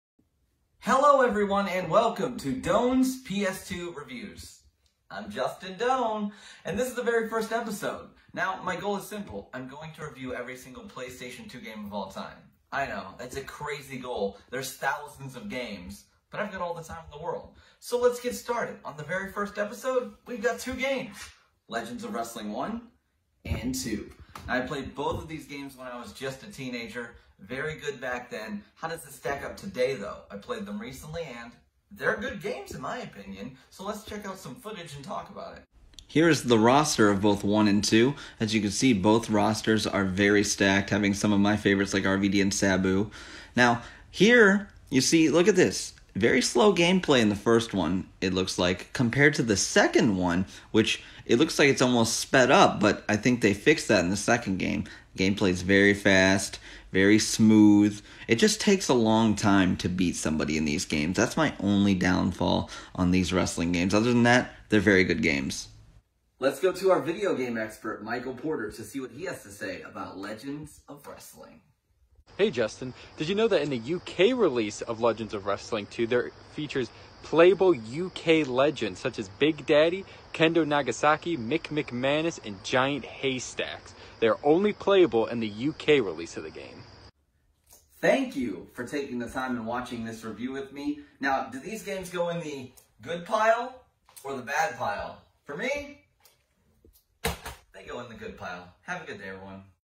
Ep. 1 - Legends of Wrestling 1 & 2
In our first episode, we review Legends of Wrestling 1 & 2!